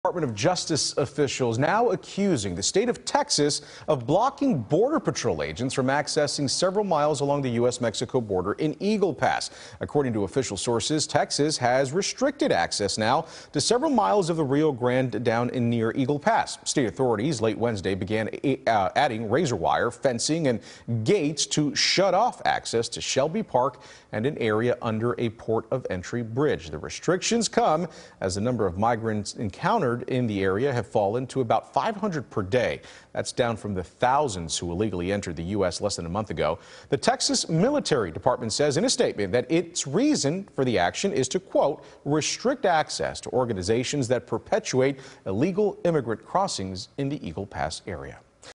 DOJ says Texas is blocking Border Patrol agents from Southern border
Department of Justice officials are accusing the state of Texas of blocking Border Patrol agents from accessing several miles of the Rio Grande along the U.S.-Mexico border in Eagle Pass. State authorities late Wednesday began adding razor wire fencing and gates to shut off access to Shelby Park, which is an area under a port of entry bridge.